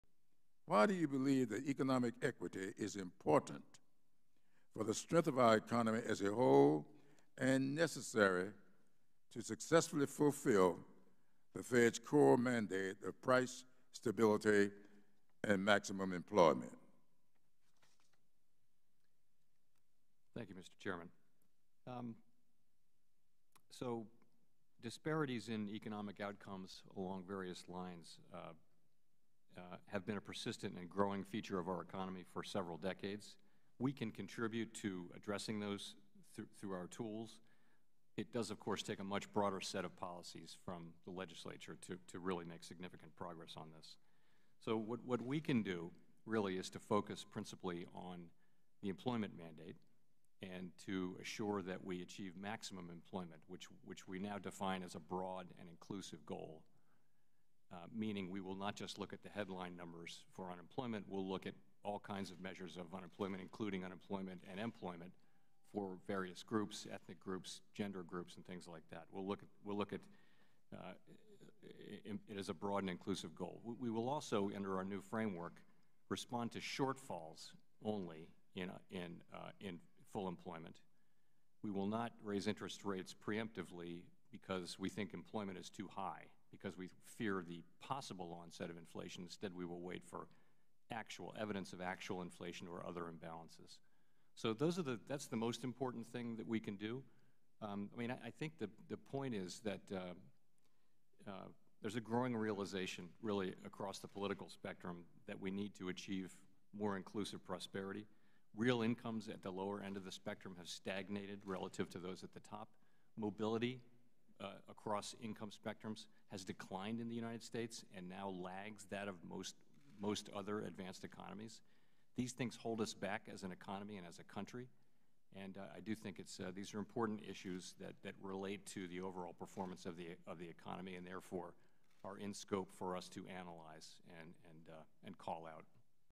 Jay Powell Says The Fed Won't Raise Rates Preemptively
Jun.22 -- Federal Reserve Chairman Jerome Powell says the best way for the Fed to reduce inequality is to focus on jobs. He speaks in response to a question before the House Select Subcommittee on the Coronavirus Crisis.